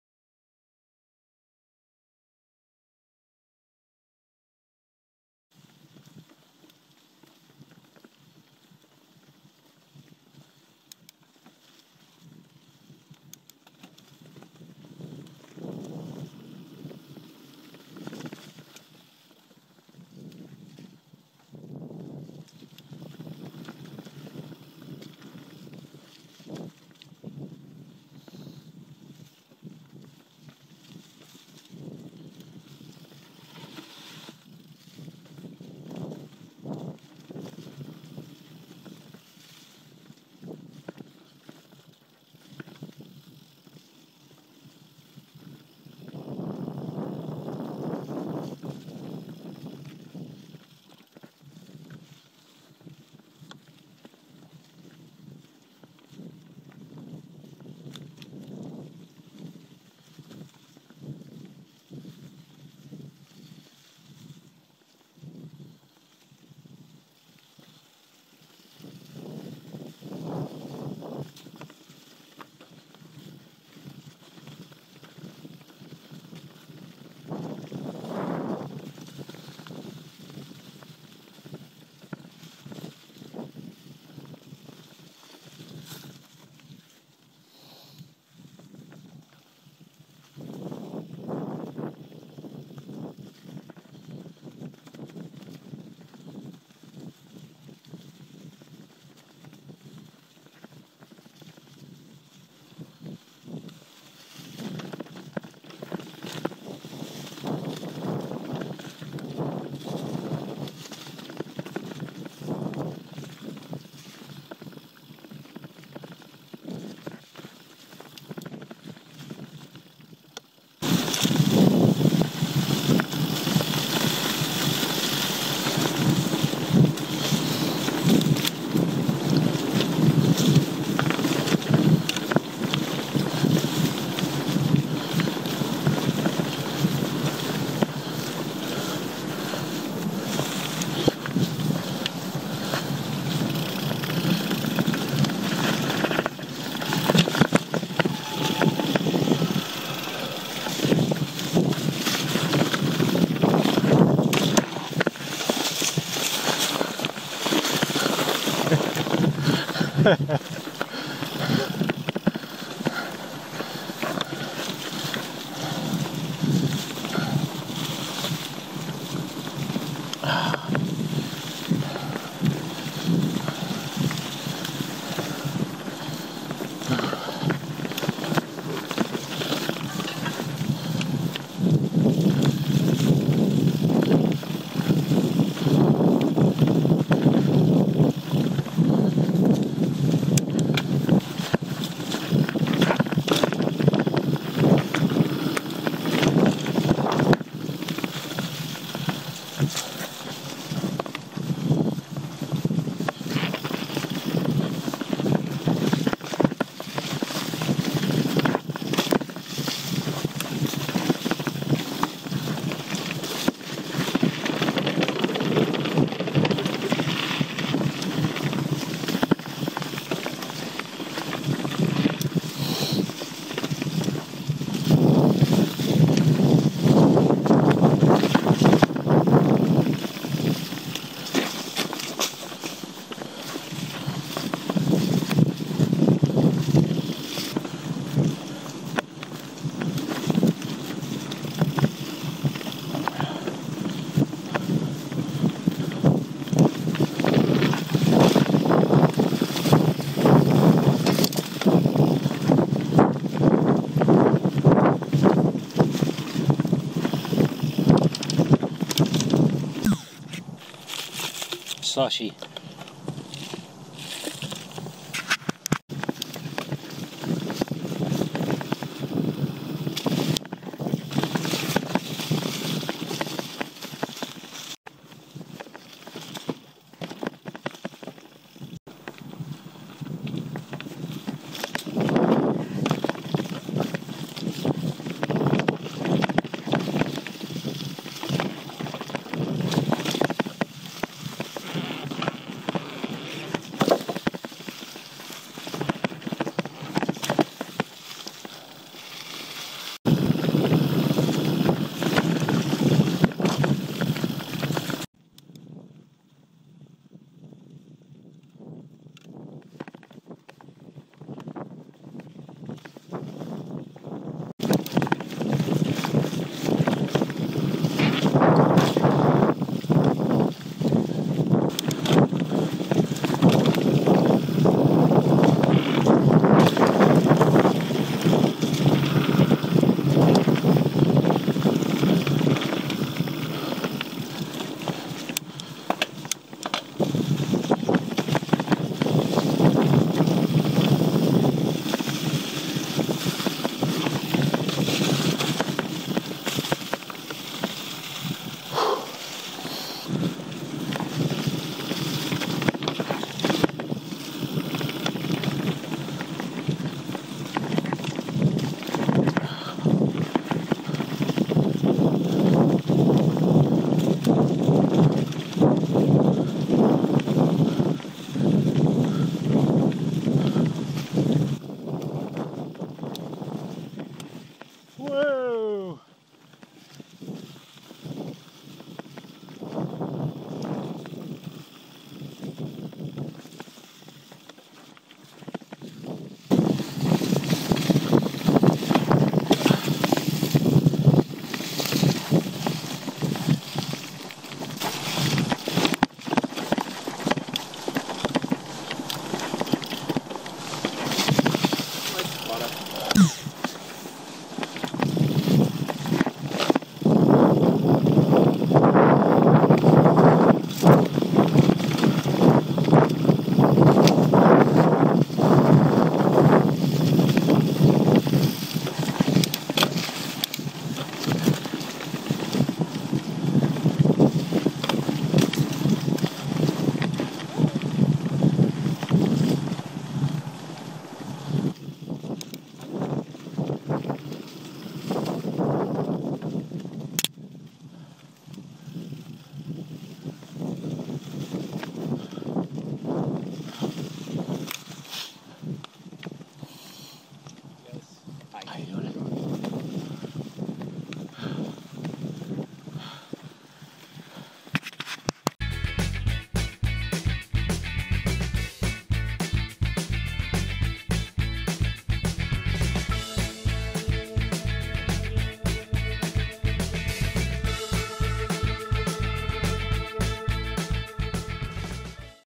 Perfect Winter Ride @ North Bethlehem
Bobby and I get great conditions for a Dec day in the North East.